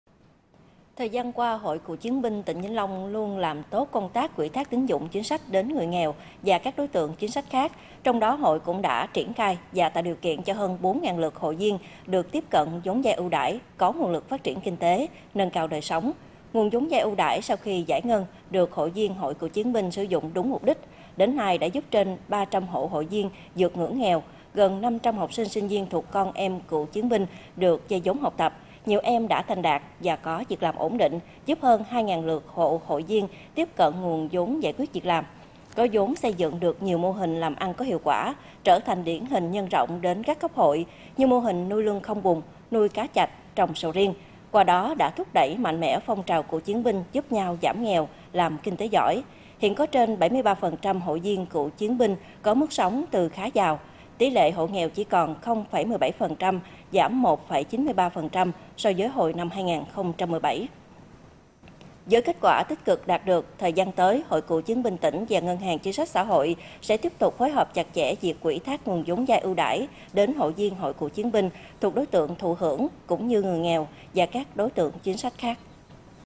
Hơn 4000 lượt hội viên CCB đã tiếp cận tín dụng chính sách
Thời gian qua Hội Cựu chiến binh tỉnh Vĩnh Long luôn  làm tốt công tác ủy thác tín dụng chính sách đến người nghèo và các đối tượng chính sách khác. Trong đó, hội cũng đã triển khai và tạo điều kiện cho hơn 4000 lượt hội viên được tiếp cận vốn vay ưu đãi, có nguồn lực phát triển kinh tế, nâng cao đời sống.

⬇️⬇️⬇️ Tải THVL Audio để nghe đầy đủ